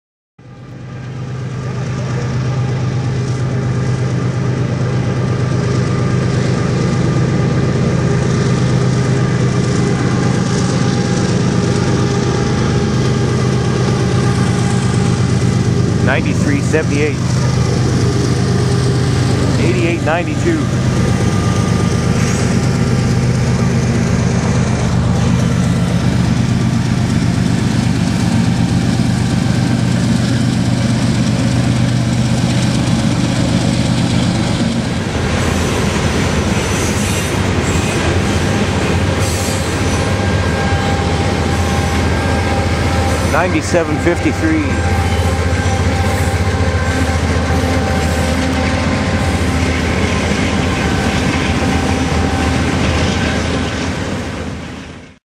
CP Train Spotting: CP 9378 8892 & 9753 Leads Late Night Coal At Revelstoke BC 23/24 7/26/12
Train ID: N/A
Power: CP ES44AC 9378 ES44AC 8892 AC44CW 9753
Location: Revelstoke, BC
Milepost: N/A
Subdivision: Shuswap
Extra Notes: N/A
Date And Time: 11:29 & 11:31PM 7/26/12
Other Railfans (Total Catches: 99)